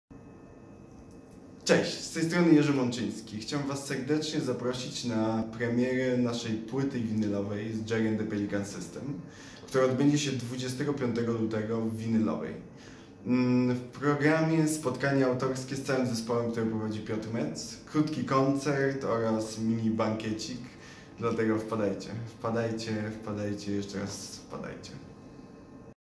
Jerzy Mączyński zaprasza na spotkanie do Winylowej !!!
Premiera płyty Winylowej Jerry & The Pelican System
Winylowa ul. Gustawa Morcinka 5 lokal 17
Start spotkania : 20:00